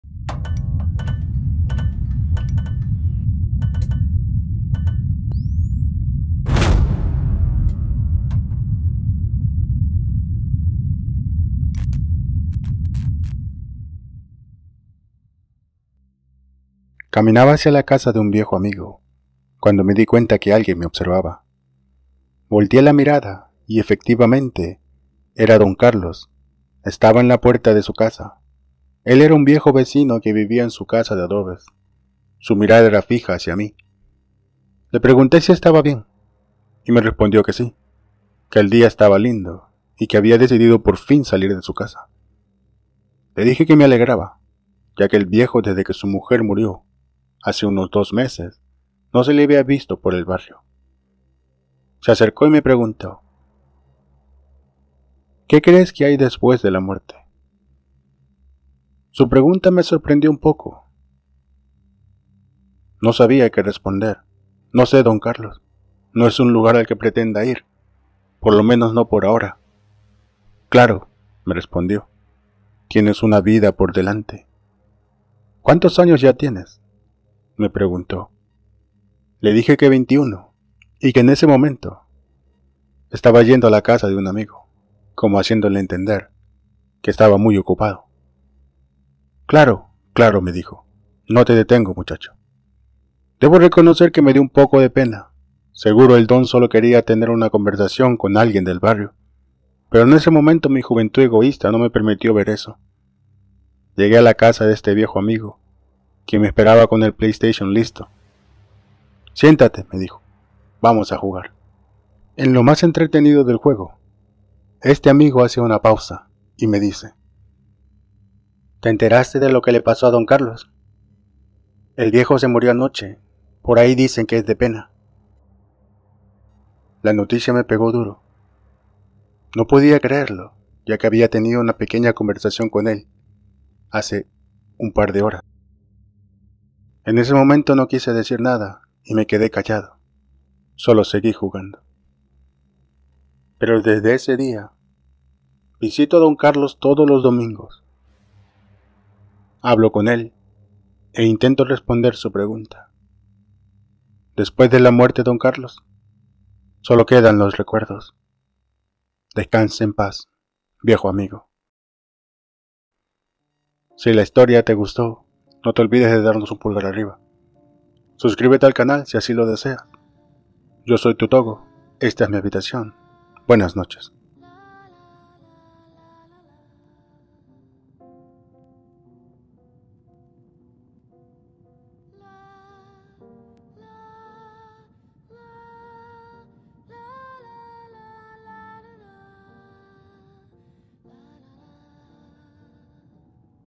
Historias reales: Hasta nunca
Despues de la muerte de su esposa, Don carlos no se la habia visto por el barrio, cuando un dia lo encuentro en la puerta de su casa y tenemos una pequeña conversacion..